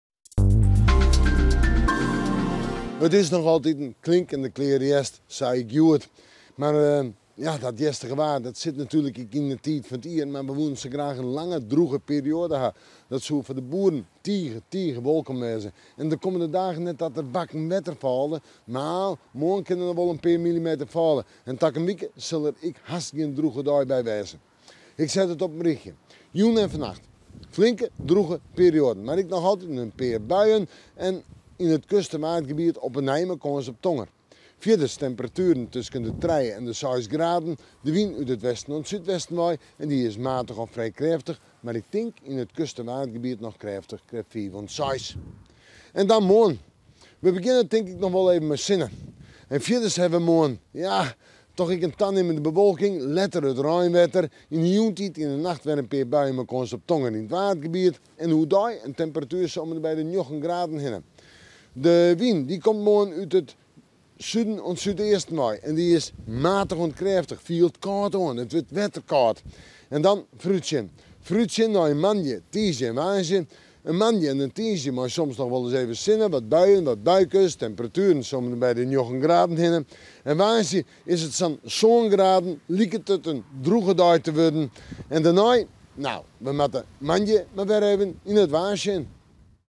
It waar fan sneon 3 novimber 2012
It wurdt in sneon mei yn 'e rin fan de moarntiid inkelde buien. De sinne kommt út en troch ek foar 't ljocht, De temperatueren komme te lizzen om de 9 of 10 graden by in matige oant krêftige wyn út it suden oant súdwesten. Der is kâns op tonger.

Foarútsjoch

Sneon krije wy yn de rin fan de dei út it súdwesten wei wer inkelde buien, mar ek geregeld sinneskyn. Maksimaal 9 of 10 graden. Snein wurdt in aardige dei mei droech waar of heechút in bui yn it Waadgebiet en 9 graden. Kommende wike komt opnij it hjerstige waar mei rein en buien werom, by 9 graden.